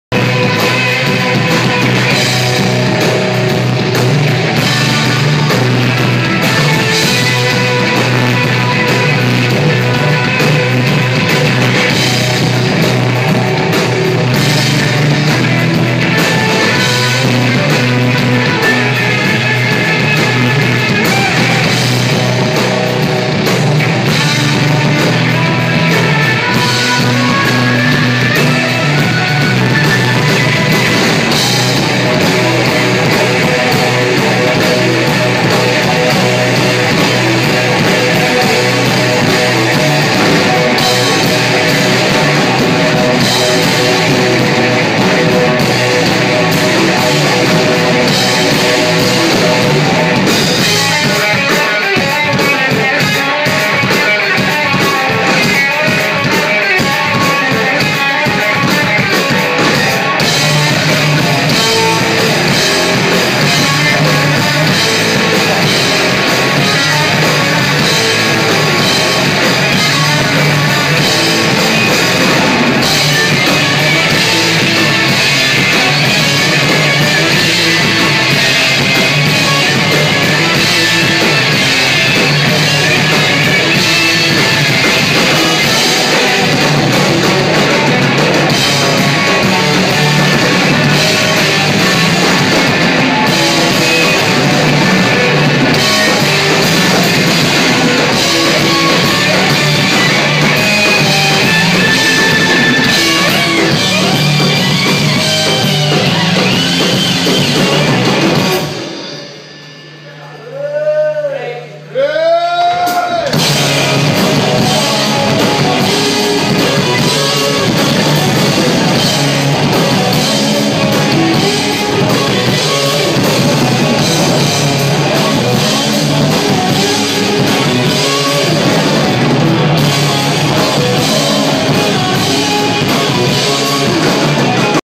HUMAN FLY / Live in Doncaster
A short excerpt of the incredible HUMAN FLY / Live in Doncaster 25th October 2013.